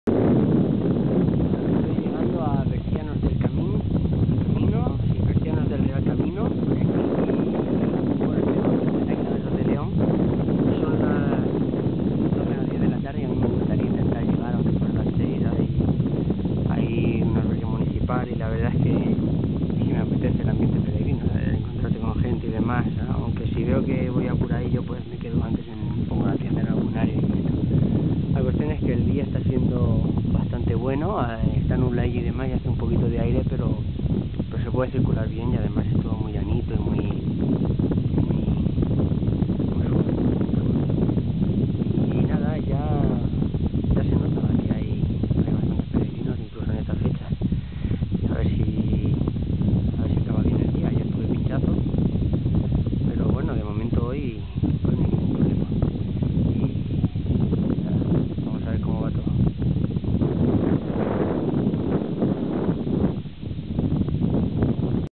JAKOBSWEG. TODO VA BIEN. CAMINO DE SANTIAGO XACOBEO 2010.
Pequeña parada en una mañana en la que no había tenido ningún percance.